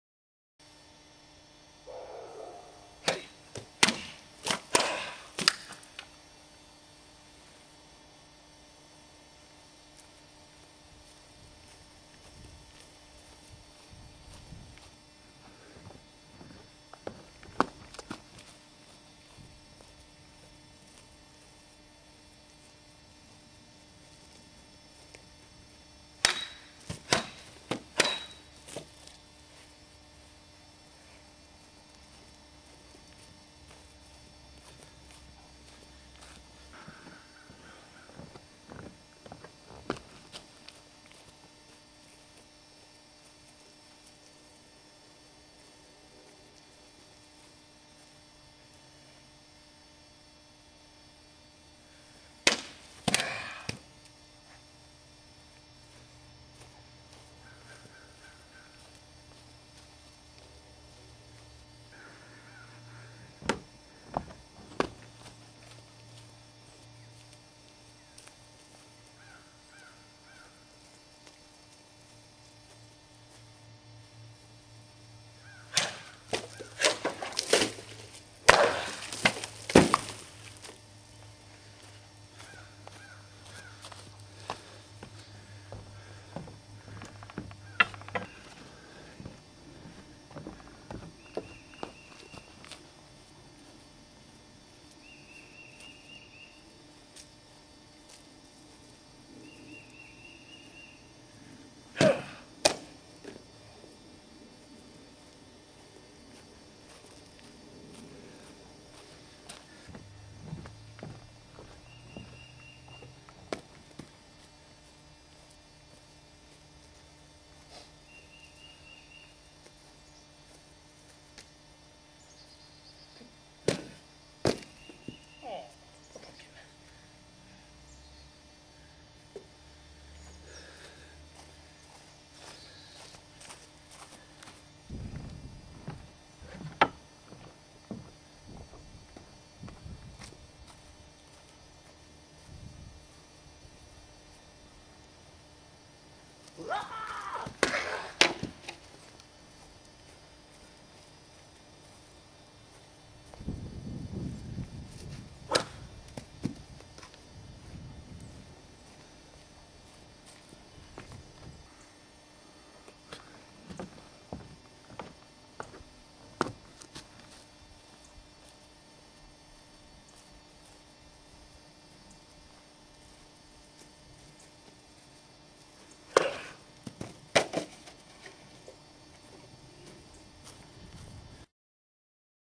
FableBlades -- Elven Saber & Dirk
Had some fun cutting with my Custom Fableblades. I've had better cutting sessions with these swords, but I was just having a bit of fun this time, left the bad cuts in.